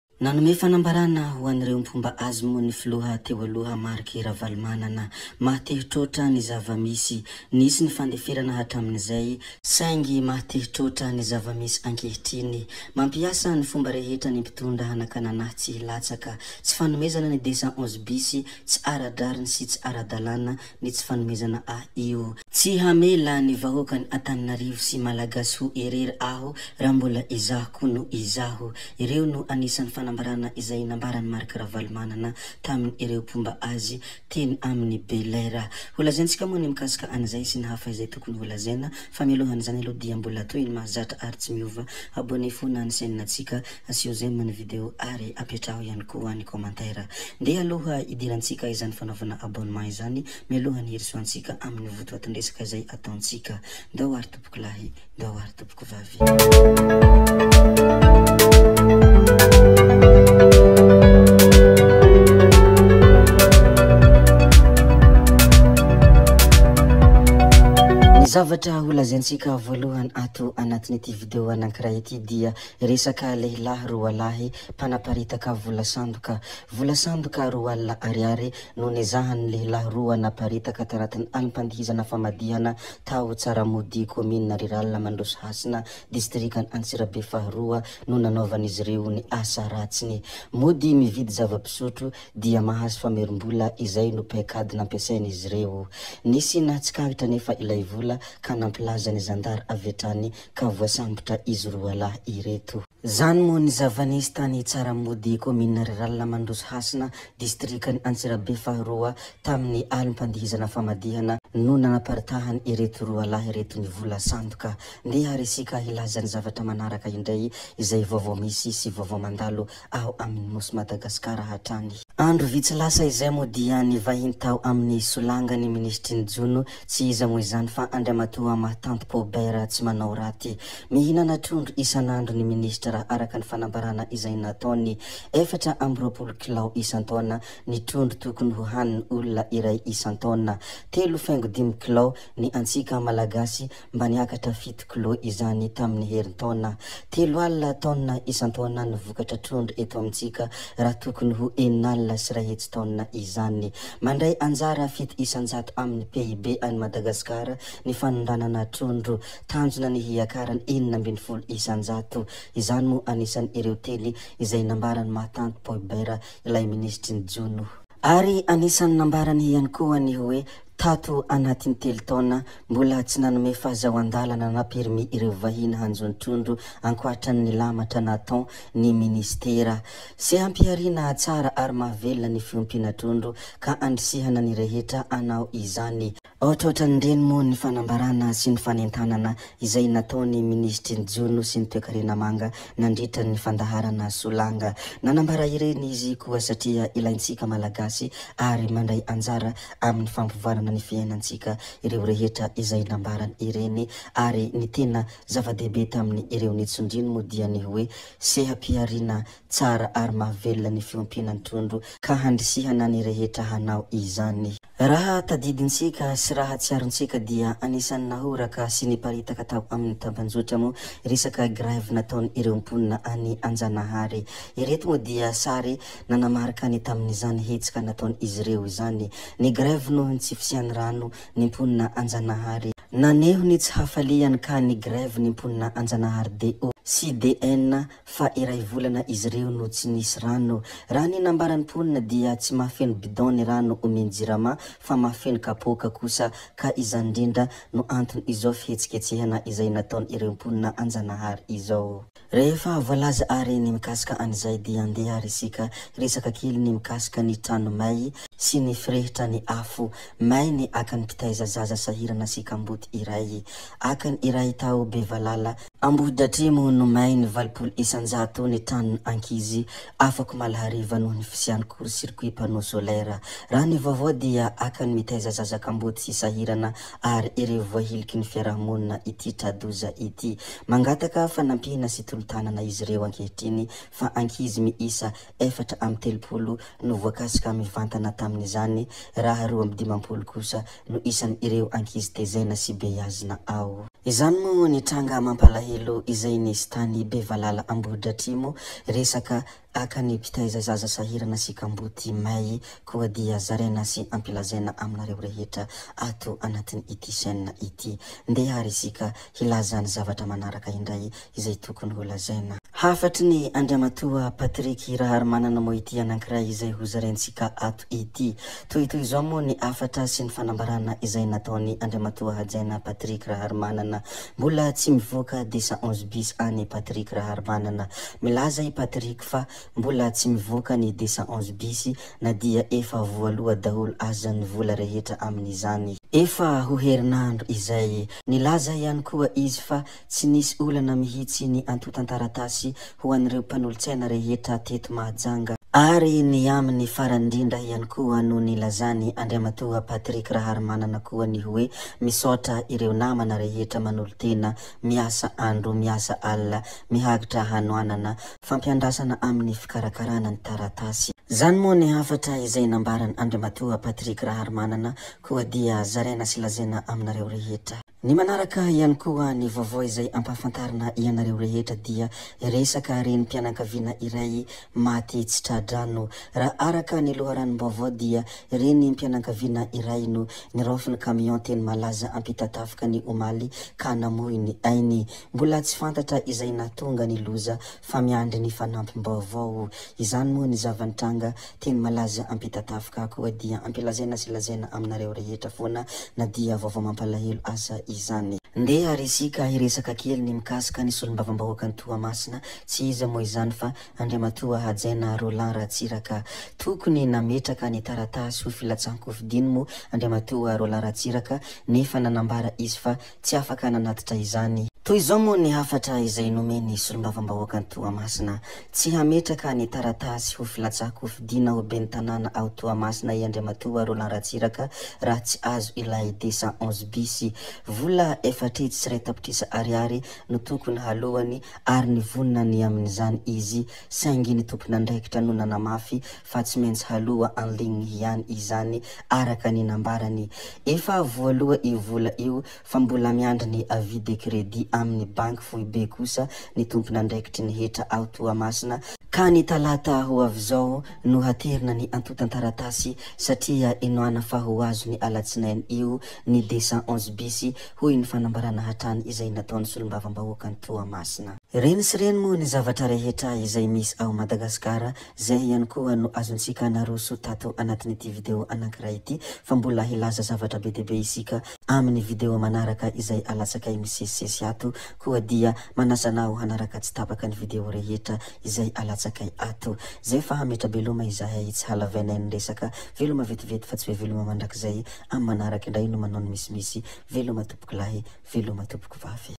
NISY NY FANDEFERANA HATRAMINY IZAY HOY RAVALOMANANA ( Nouveautés gasy 2024 )
MAHA TE HITRAOTRA NY ZAVA-MISY HOY FILOHANY TEO ALOHA MARC RAVALOMANANA RAHA NILAZA SY NANAMBARA TAMINY MPANARADIA SY MPOMBA AZY. ‘ISY NY FANDEFERANA HATRAMIZAY SAINGY MAHA TE HITRAOTRA NY ZAVA-MISY ANKEHITRINY HOY IZY . MAMPIASA NY FOMBA REHETRA NY MPITONDRA HANAKANANA AHY TSY HILATSAKA HOY IZY . FA IZAHO KOSA TSY HAMELA NY VAHOAKANY ANTANANARIVO HO IRERY SY NY MALAGASY HO IRERY RAHA MBOLA IZAHO KO NO IZAHO HOY HAFATRA IZAY NOLAZAINY . MANASA ANAO ARY HANARAKA HATRAMINY FARANY ITY VIDEO ITY.